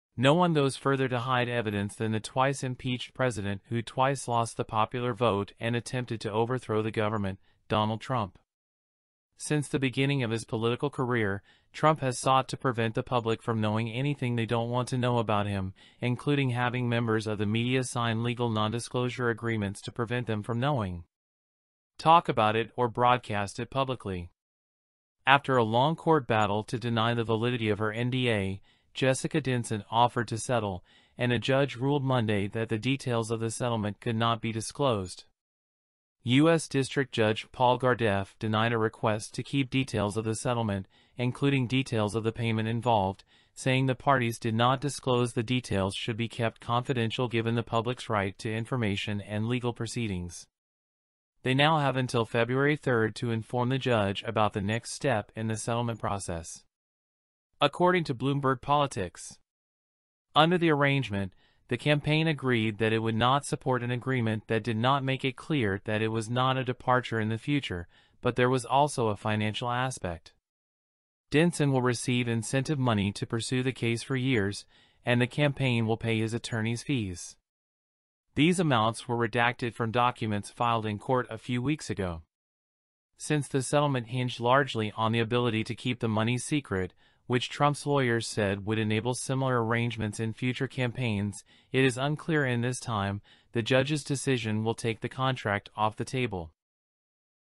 US District Judge Rules Against Trump Campaign In NDA Case
No one goes further to hide evidence than the twice impeached president who twice lost the popular vote and attempted to overthrow the government, Donald Trump. Since the beginning of his political career, Trump has sought to prevent the public from knowing anything they don't want to know about him, including having members of the media sign legal non-disclosure agreements to prevent them from knowing. talk about it or broadcast it publicly. After a long court battle to deny the validity of her NDA, Jessica Denson offered to settle, and a judge ruled Monday that the details of the settlement could not be disclosed.